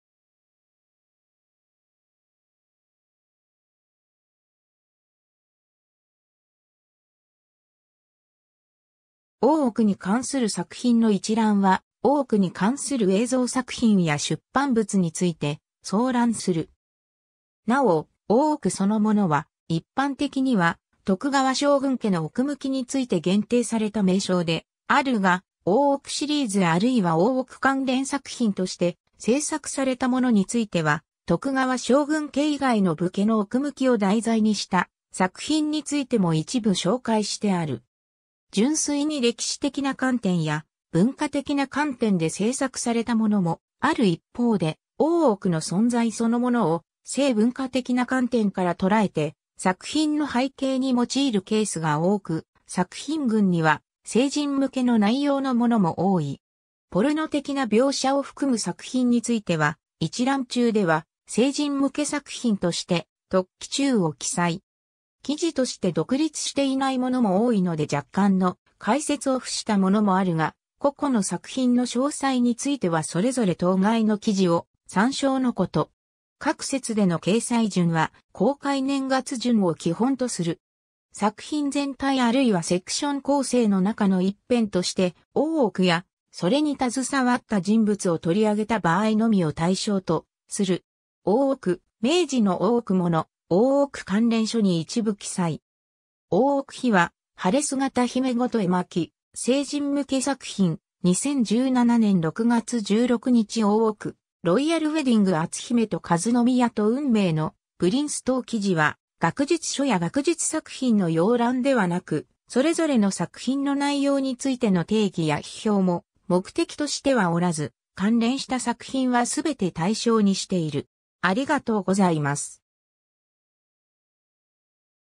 大奥に関する作品の一覧
大奥に関する作品の一覧（おおおくにかんするさくひんのいちらん）は、大奥に関する映像作品や出版物について総覧する。
 なお、「大奥」そのものは一般的には徳川将軍家の奥向きについて限定された名称である（詳細は大奥の記事を参照の事）が、大奥シリーズあるいは大奥関連作品として製作されたものについては、徳川将軍家以外の武家の奥向きを題材にした作品についても一部紹介してある。
 純粋に歴史的な観点や文化的な観点で製作されたものもある一方で、大奥の存在そのものを性文化的な観点から捉えて作品の背景に用いるケースが多く、作品群には成人向けの内容のものも多い。
ポルノ的な描写を含む作品については、一覧中では[※成人向け作品]として特記注を記載。
 記事として独立していないものも多いので若干の解説を付したものもあるが、個々の作品の詳細についてはそれぞれ当該の記事を参照の事。
 各節での掲載順は公開年月順を基本とする。
 ※作品（番組・書籍）全体あるいはセクション構成の中の一編として大奥やそれに携わった人物をとりあげた場合のみを対象とする。
（単に話題として触れたり会話の中で登場したりする場合や、クイズ形式で出題されただけの場合などは対象外。） ※大奥#明治の大奥もの、大奥#関連書に一部記載。
 大奥秘話・晴姿姫ごと絵巻　(1974年1月)　[※成人向け作品] （出演者は大奥に関連した役） ○2017年6月16日 大奥ロイヤルウェディング　篤姫と和宮と運命のプリンス ^ 当記事は学術書や学術作品の要覧ではなく、それぞれの作品の内容についての定義や批評も目的としてはおらず、関連した作品はすべて対象にしている。